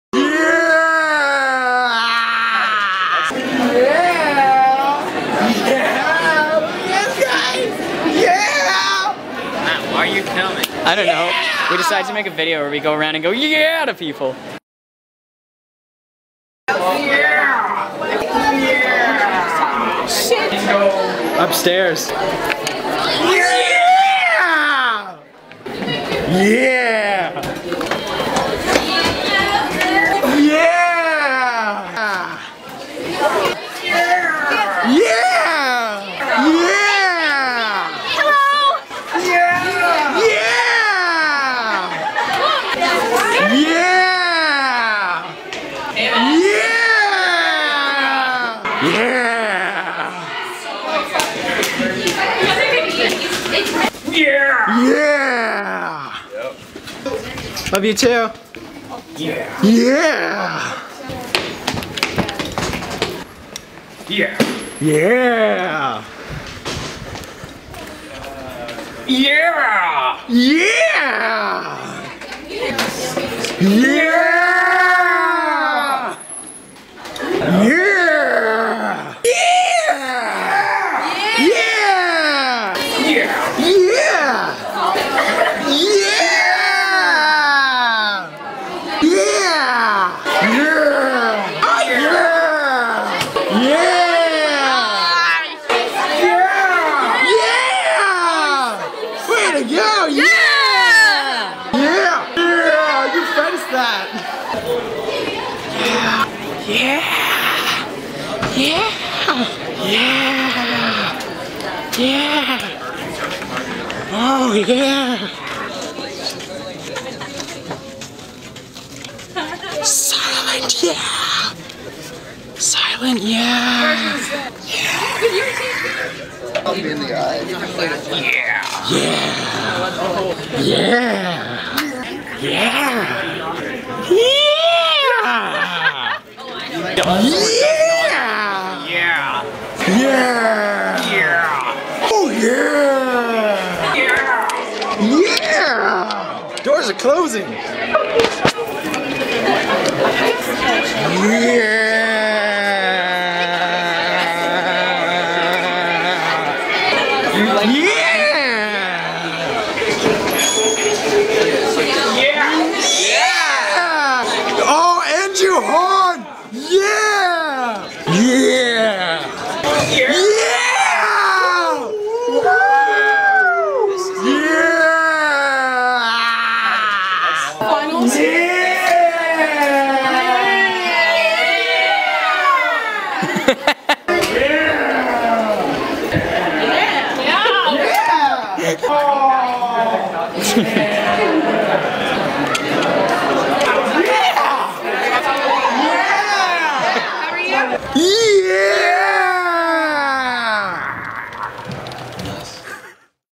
yeah!!!
A video in which me and my friend walk around saying "Yeah."